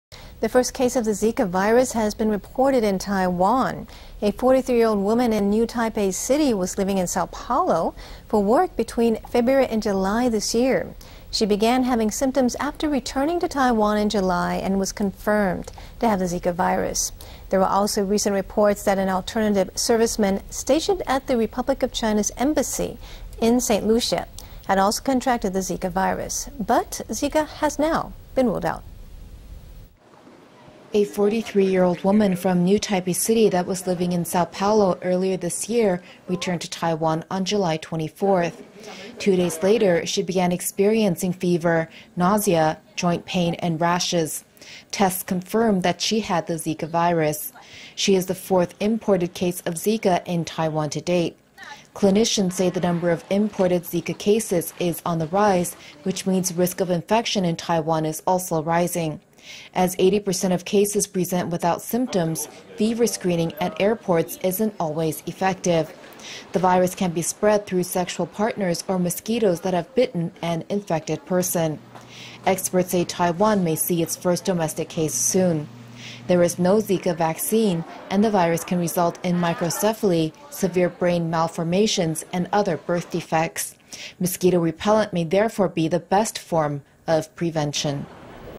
首例！ 國人感染茲卡病毒個案 First case of Ziak virus in Taiwan —宏觀英語新聞
The first case of the Zika virus has been reported in Taiwan.  A 43-year-old woman in New Taipei City was living in Sao Paolo for work between February and July this year.  She began having symptoms after returning to Taiwan in July and was confirmed to have the Zika virus. There were also recent reports that an alternative serviceman stationed at the Republic of China's embassy in St. Lucia had also contracted the Zika virus, but Zika has now been ruled out.

出現首例國人感染茲卡病毒的個案！是一名住在新北市的43歲女性，今年2月到7月、陪同先生到聖露西亞工作，上個月回國後出現症狀，確定感染茲卡病毒。另外還傳出我駐聖露西亞大使館，也有1名替代役男感染茲卡病毒，不過目前已經排除。